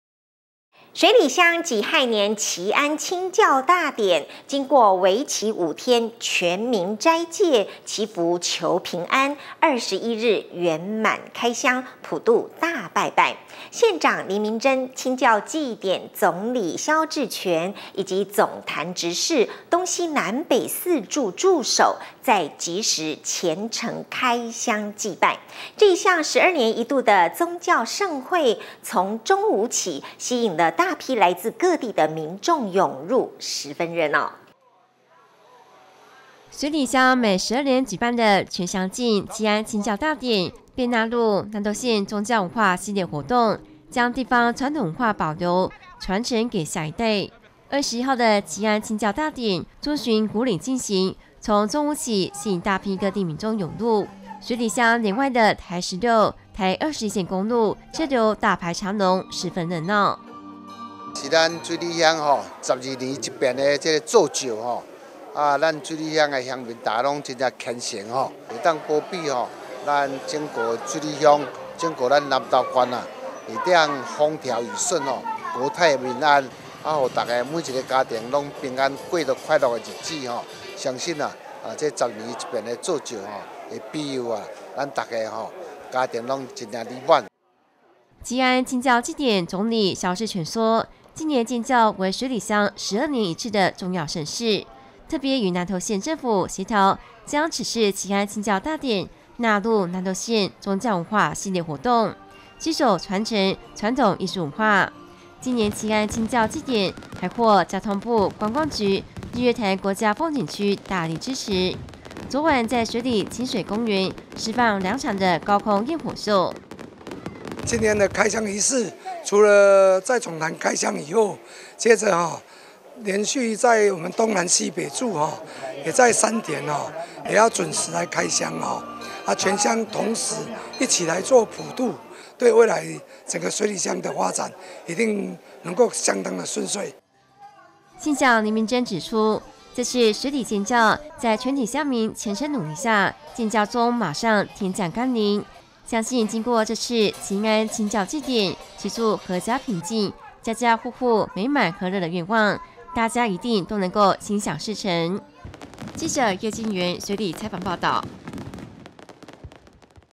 水里祈安清醮祭典開香 林縣長主持祈福歡慶
水里鄉己亥年祈安清醮大典，經過為期5天全民齋戒祈福求平安，21日圓滿開香普渡大拜拜。縣長林明溱、清醮祭典總理蕭志全及總壇執事、東西南北四柱柱首，在下午吉時虔誠開香祭拜。這項十二年一度的宗教盛會，從中午起吸引大批各地民眾湧入，十分熱鬧。